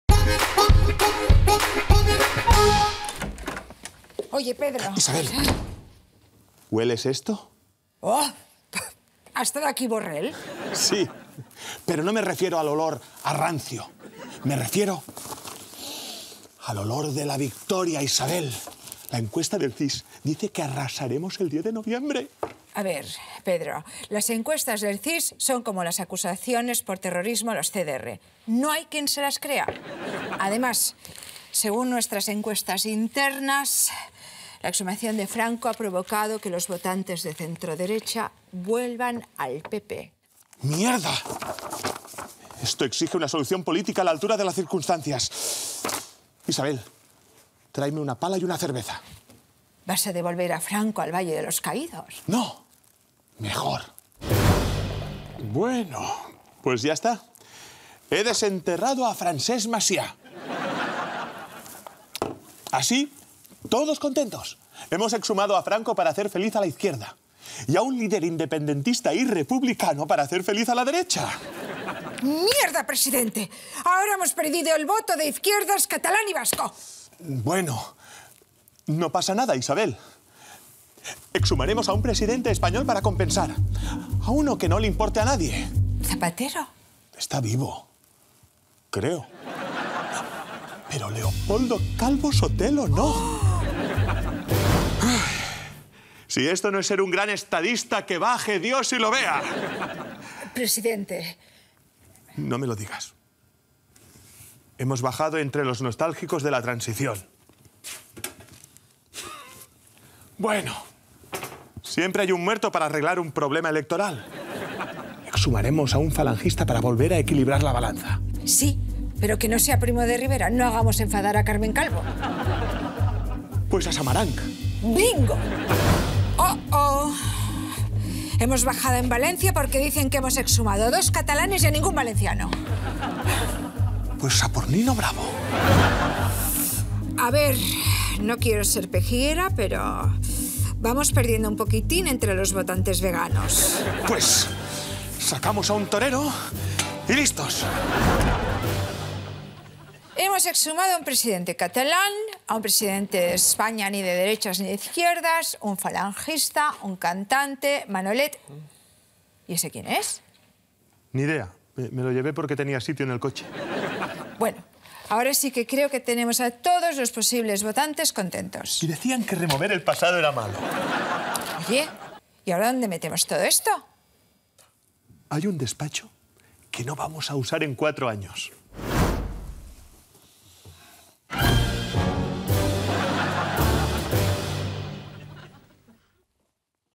Exhuma como puedas
Tot i els bons resultats al CIS, Pedro Sánchez (Pep Plaza) no les té totes de cara a les eleccions. Així que aprofitarà que encara no ha tornat la pala de desenterrar Franco per continuar exhumant cadàvers amb fins electorals.